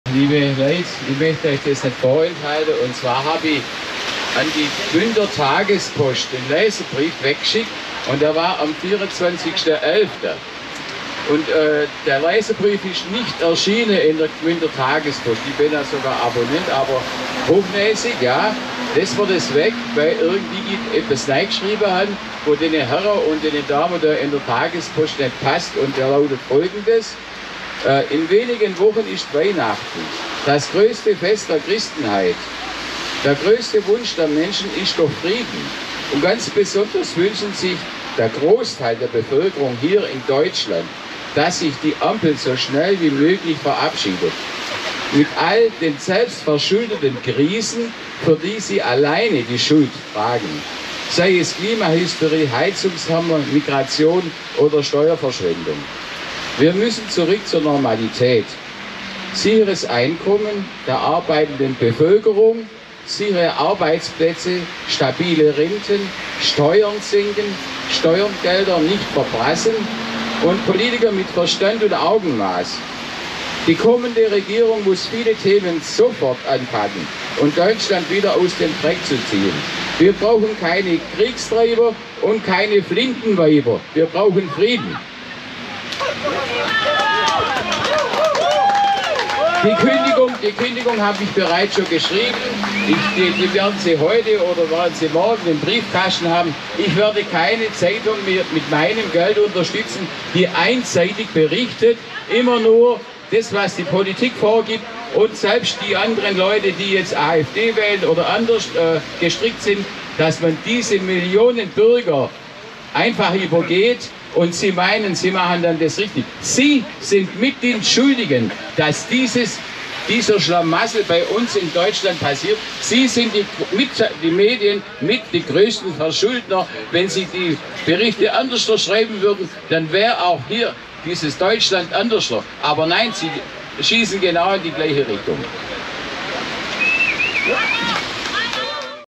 Leserbrief wurde bei der Gmünder Tagespost NICHT veröffentlicht - Schwäbisch Gmünd 12.12.2023
Bericht über einen NICHT veröffentlichen Leserbrief!
Ein Leserbrief an die Gmünder Tagespost in Schwäbisch Gmünd - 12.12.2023